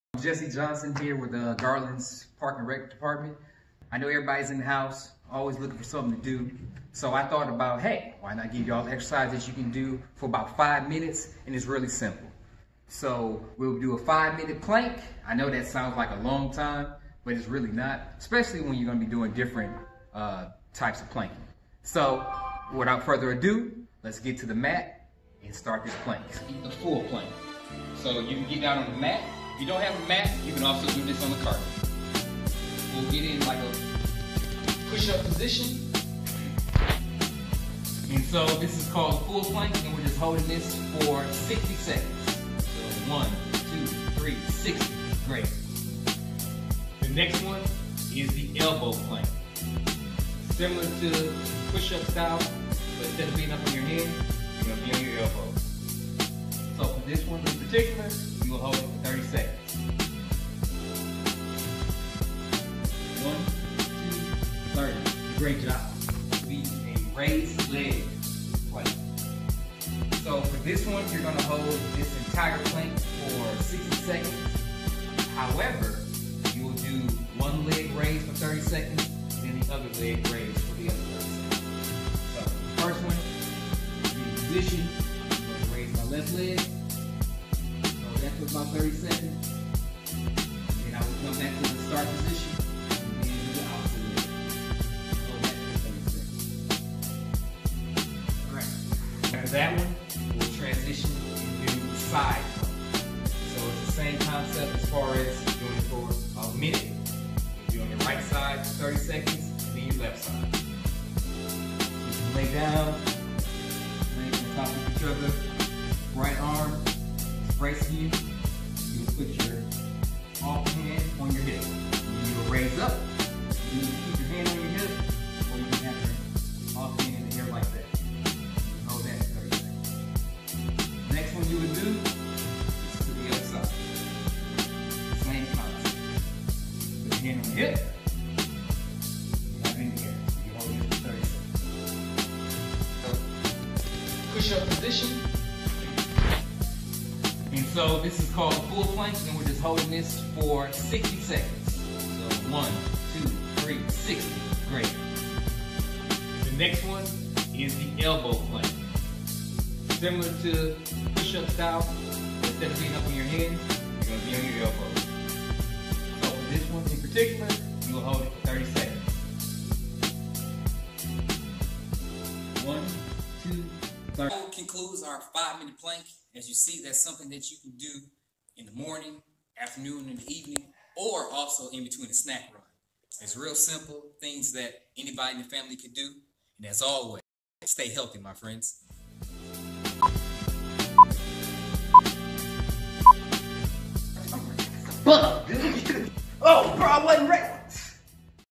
Stay & Play: 5 Minute Plank
Today's Stay & Play activity comes from Jesse from Audubon Rec Center who teaches you a great workout in five short minutes.  Check out this plank workout video and show some love for Audubon Rec Center in the comments below.